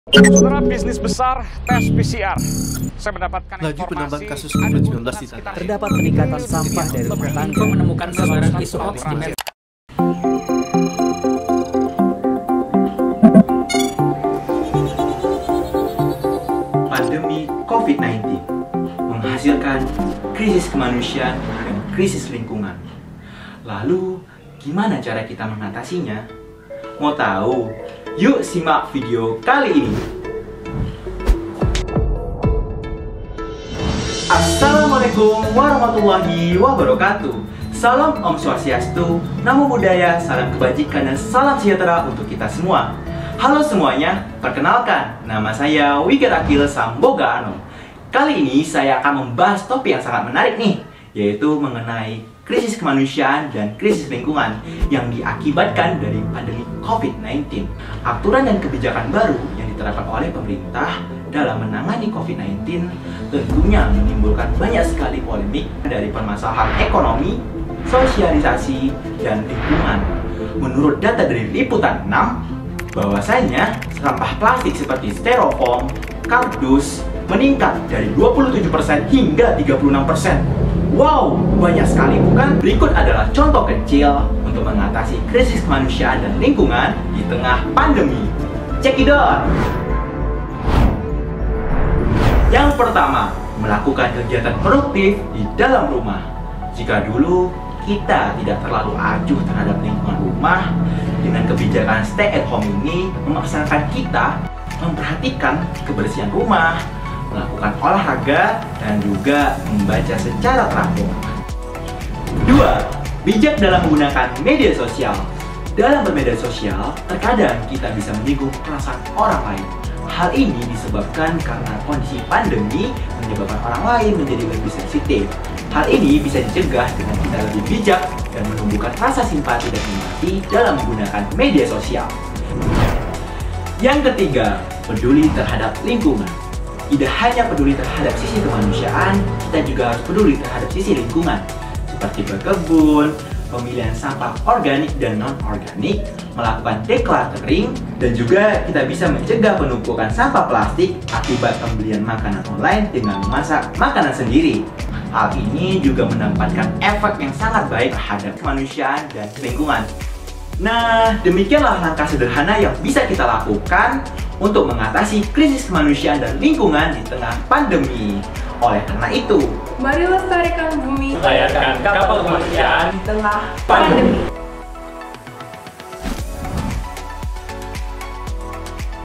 [Manusia Berbudi, Bumi lestari] NCC2021
Halo semuanya, perkenalkan nama saya Wigar 'Aqil Samboga Anom.
Kali ini saya akan membawakan topik yang sangat menarik nih, yaitu mengenai isu kemanusiaan dan isu lingkungan yang terjadi akibat pandemi Covid-19!

Pandemi Covid-19 sudah terjadi selama dua tahun terakhir dan memberikan dampak signifikan terhadap masyarakat maupun pemerintah.

Aturan serta kebijakan baru yang ditetapkan oleh pemerintah untuk mengatasi Covid-19 menimbulkan pro dan kontra. Hal ini karena kita butuh adaptasi terhadap kondisi pandemi. Kondisi tersebut menegaskan bahwa pandemi Covid-19 sangat berpengaruh dalam memicu krisis kemanusiaan dan lingkungan. Maka, diperlukan dukungan dan kerja sama dalam menghadapinya.

Oleh karena itu...
“Mari lestarikan bumi, layarkan kapal kemanusiaan di tengah pandemi”.

Gambar source by google non copyright